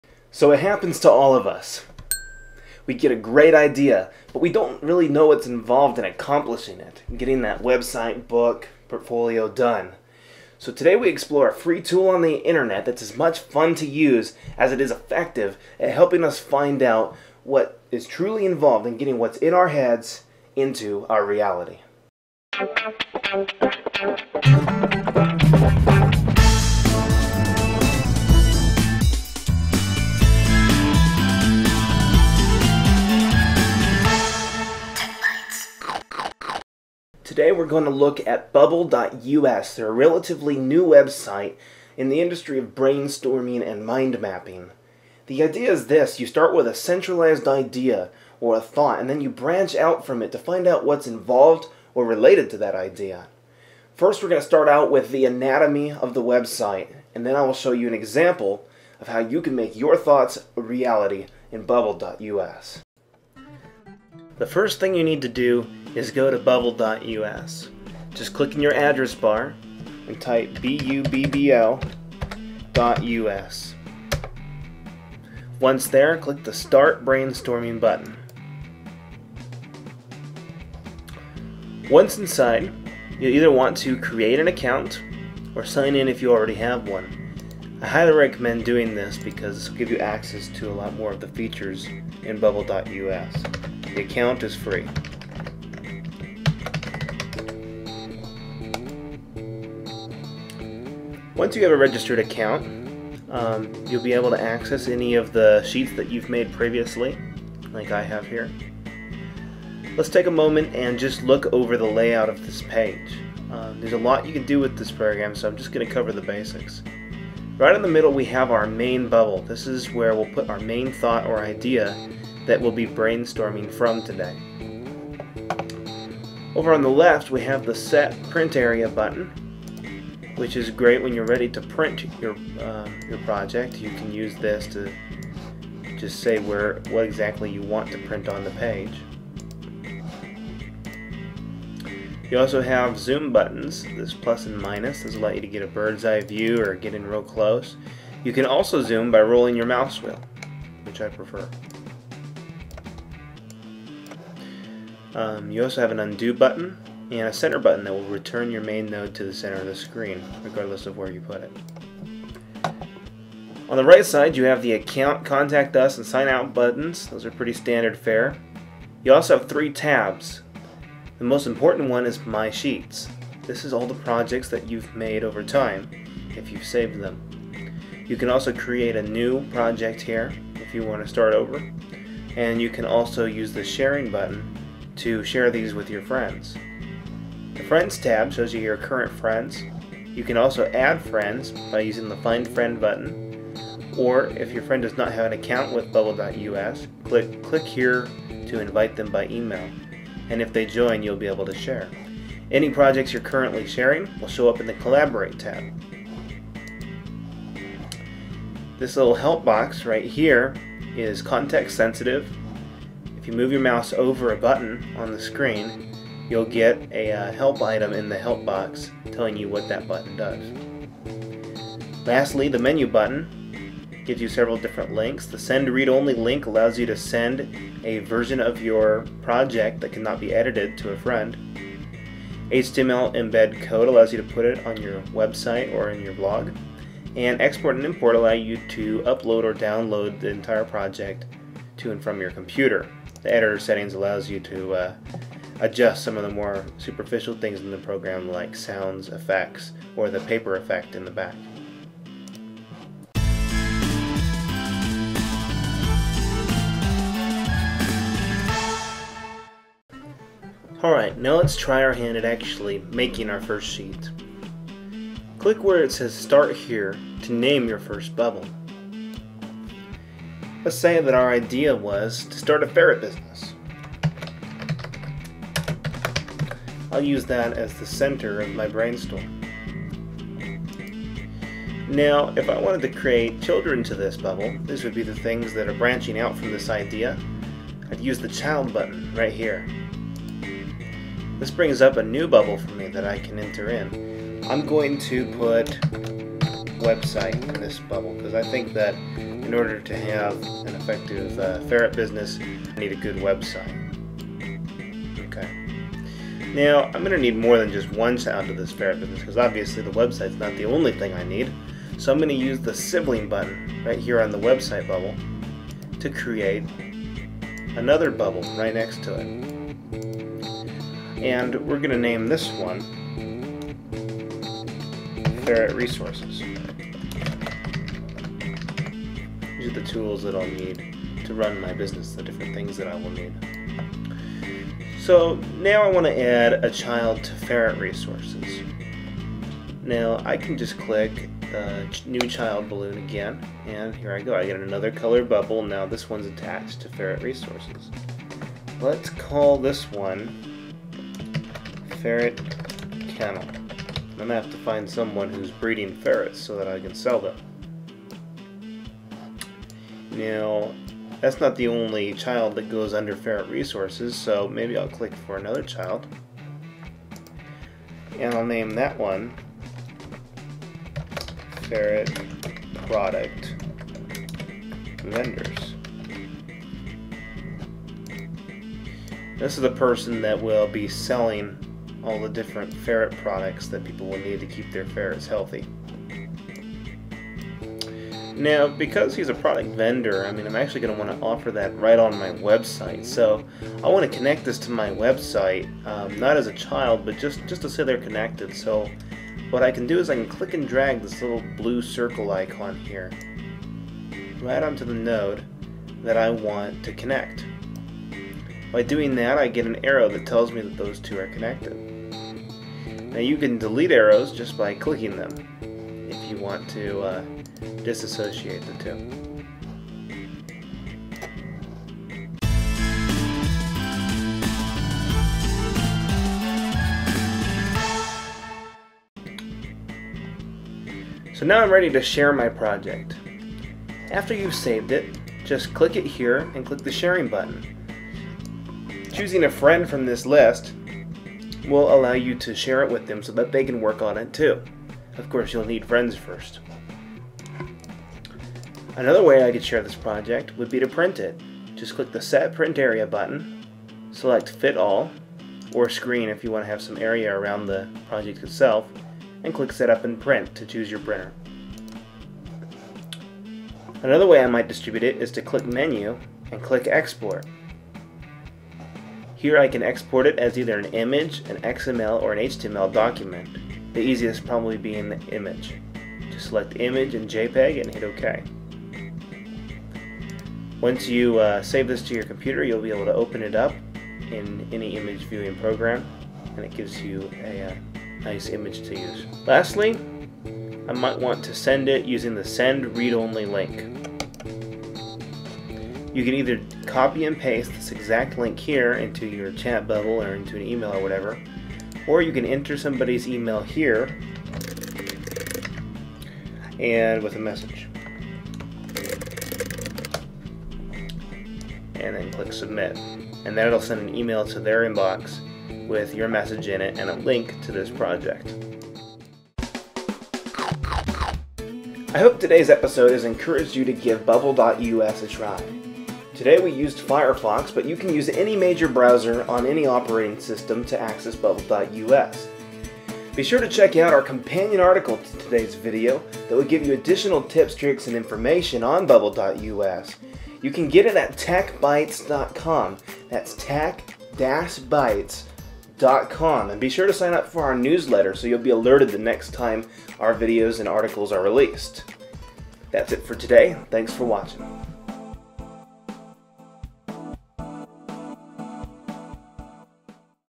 Bubbl.us Basics: Brainstorming and Mind-mapping Online
Learn the basics for Mind-mapping and Brainstorming with Bubbl.us, a free and easy-to-use web app. Learn to make your first Bubbl.us brainstorm in this free video tutorial.